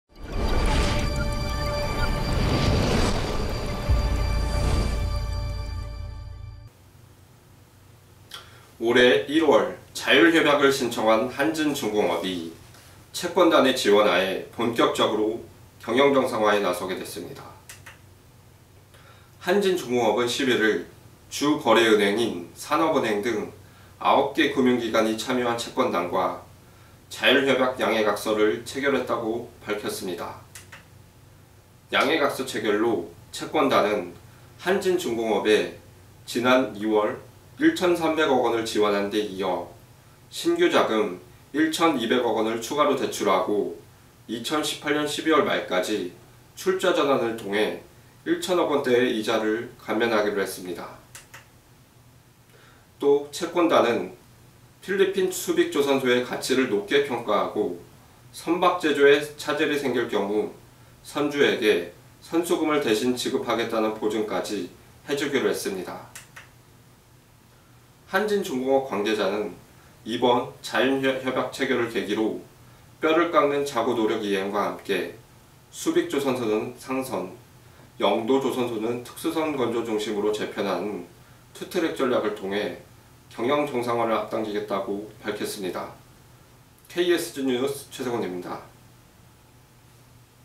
한진重, 채권단과 자율협약···경영정상화 시동
올해 1월 자율협약을 신청한 한진중공업이 채권단의 지원 하에 본격적으로 경영 정상화에 나서게 됐습니다.

한진중공업은 11일 주거래은행인 산업은행 등 9개 금융기관이 참여한 채권단과 자율협약 양해각서(MOU)를 체결했다고 밝혔습니다.

양해각서 체결로 채권단은 한진중공업에 지난 2월 1300억원을 지원한 데 이어 신규 자금 1200억원을 추가로 대출하고 2018년 12월 말까지 출자전환을 통해 1000억원대의 이자를 감면하기로 했습니다.

또 채권단은 필리핀 수빅조선소의 가치를 높게 평가하고 선박 제조에 차질이 생길 경우 선주에게 선수금을 대신 지급하겠다는 보증까지 해주기로 했습니다.

한진중공업 관계자는 “이번 자율협약 체결을 계기로 뼈를 깎는 자구노력 이행과 함께 수빅조선소는 상선, 영도조선소는 특수선 건조 중심으로 재편하는 투트랙 전략을 통해 경영 정상화를 앞당기겠다”고 밝혔습니다.